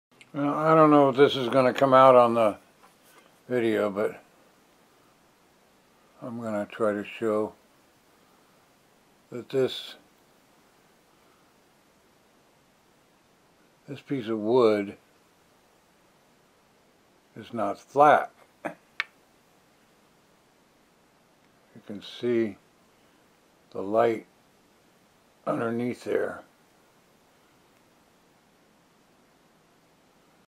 Checking wood for flatness by John A Tuttle from Player-Care.com
This is a very simple test to check a piece of wood for flatness. In this case, the block is part of a control device, and since it's not flat, air is leaking, which is causing problems. The wood is about 100 years old.